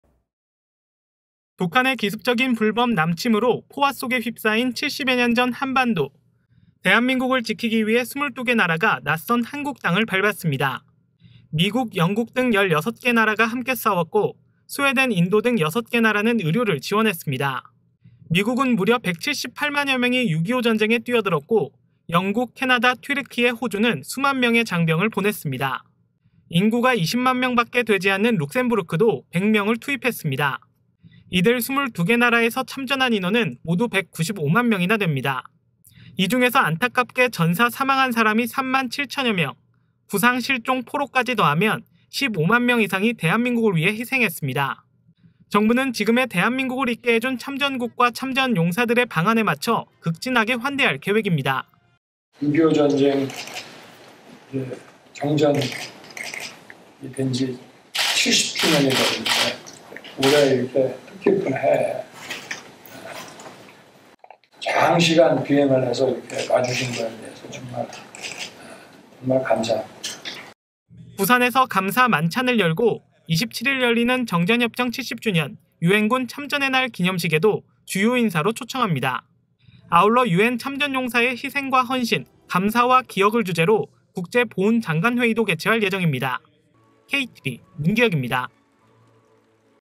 대한민국 위해 싸운 22개국…"극진한 환대로 보답"
6.25 전쟁 당시
22개 나라가
자유 대한민국을
지키기 위해서
참전했는데요.

이역만리의
전장터에 뛰어든
195만 명의
젊은이들 가운데,

전사자만도
3만7천 명이
넘었습니다.

정부는 이번에
방한한 참전국
대표단과 참전
용사들을 극진하게
환대한다는 계획입니다.